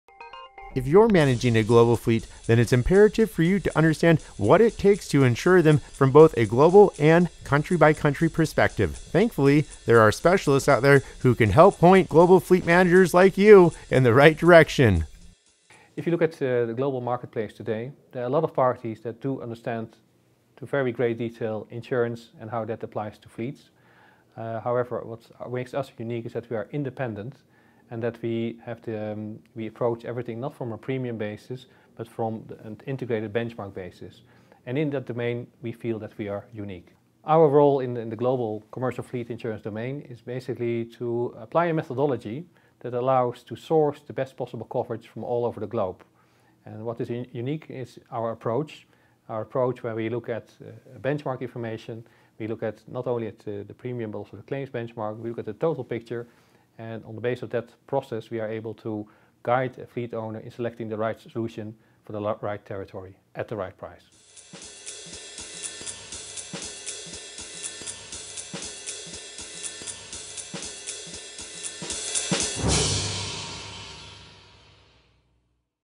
Insurance and Global Fleets | Eelco van de Wiel | Automotive Digest
If you're managing a global fleet, then it's imperative for you to understand what it takes to insure them from both a global and country-by-country perspective. Thankfully, there are specialists out there who can help point global fleet managers in the right direction.

Eelco van de Wiel
Owner
Fleet Insurance

Ain_10_23_1013_NafaIntFleetSem_FleetIns_EelcovandeWiel_VC2